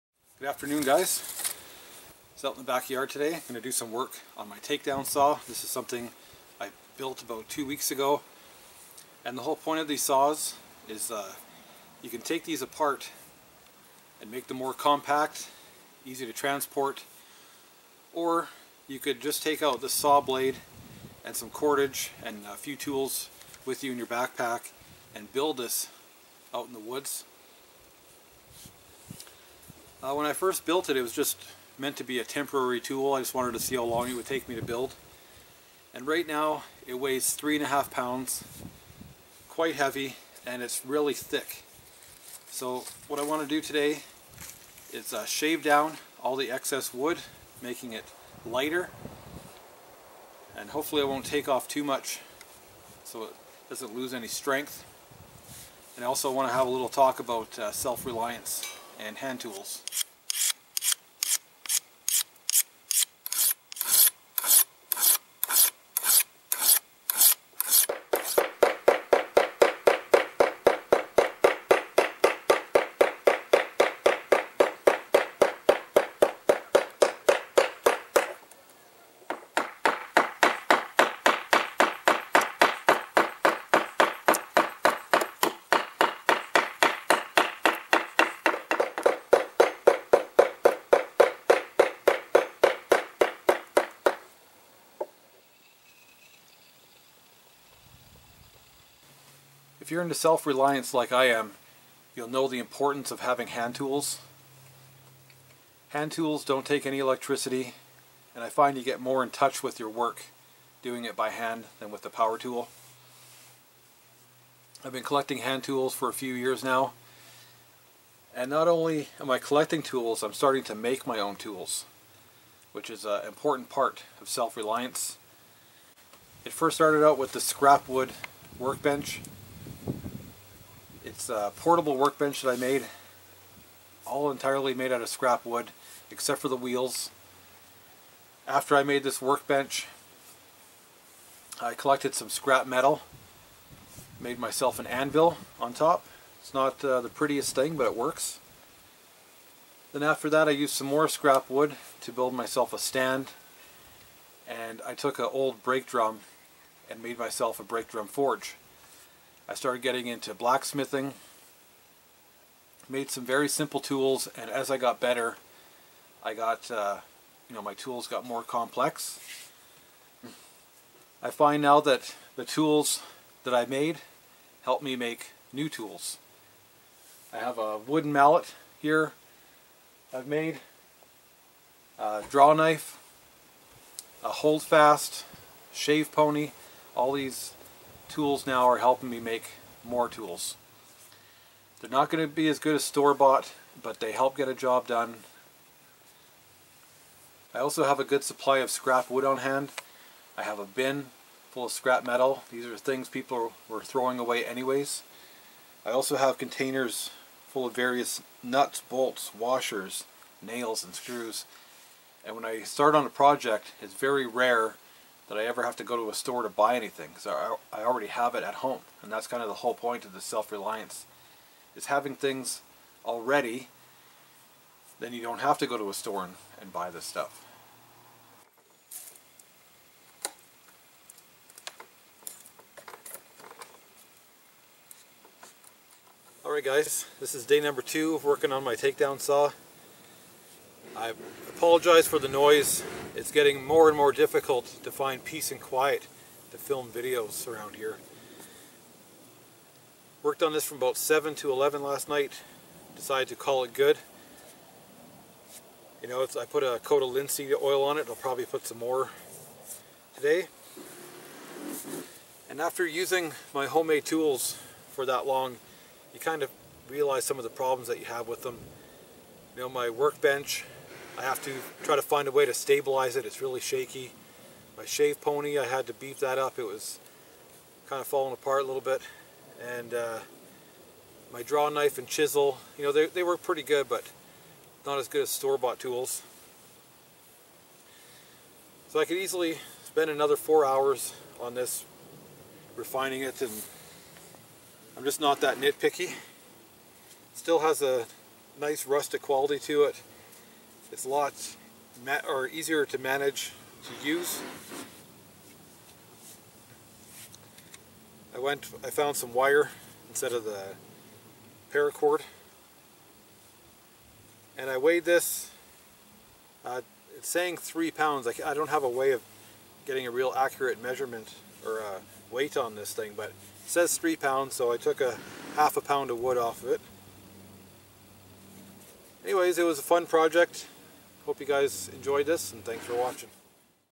Takedown Saw & Self-Reliance Tools
Description: Worked on my takedown saw using only  hand tools that I made myself.